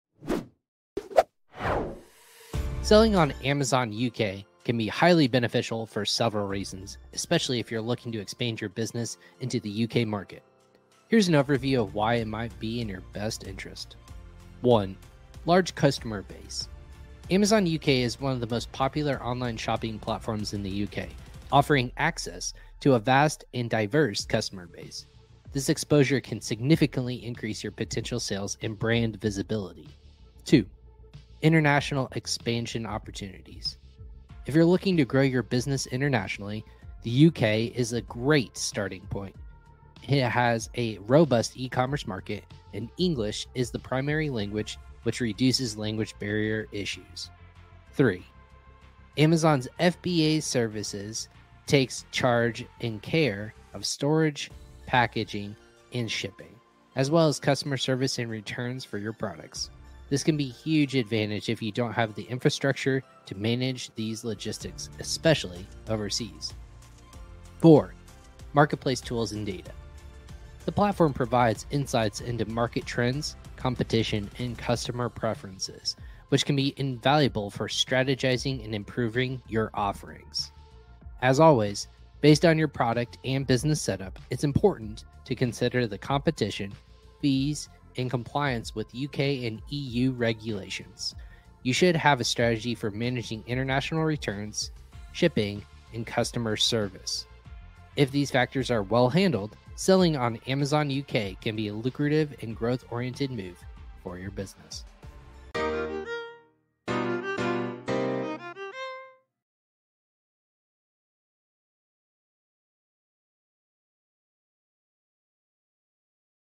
2024 Amazon UK Marketplace Overview
Thinking about selling on the Amazon UK Marketplace in 2024?! 🇬🇧

Here's what you need to know:

💻 Expansive Customer Base: Amazon UK, one of the UK's leading online shopping platforms, can dramatically boost your sales and brand visibility by tapping into its vast and diverse customer base.

📈 International Expansion Made Easy: With its robust e-commerce environment and English as the primary language, is an ideal starting point for most  international brands growing quickly.

📦 Leveraging Amazon's FBA Services: Take advantage of Amazon's Fulfillment by Amazon (FBA) services, including storage, packaging, shipping, customer service, and handling returns, which can be a game-changer for businesses lacking overseas logistics infrastructure.

🛠️ Marketplace Tools and Data Insights: Know your customer before you expand. Understand market trends, competition, and customer preferences using Amazon's advanced tools and data as well as other 3rd party software tools like Helium 10, Jungle Scout and Data Dive. This invaluable information can significantly aid in strategizing and enhancing your product offerings.

🔍 Strategic Considerations: We also delve into important aspects like managing competition, understanding fees, and complying with UK and EU regulations. Plus, get tips on handling international returns, shipping, and customer service effectively.

🌟 Whether you're a seasoned seller or new to the Amazon marketplace, it's important to make informed decisions and strategically grow your business.

Drop your questions and experiences in the comments section below – let's learn and grow together!

📆 Chapters:
0:03 - Introduction to Amazon UK's potential
0:36 - Benefits of international expansion
1:15 - Strategic considerations for success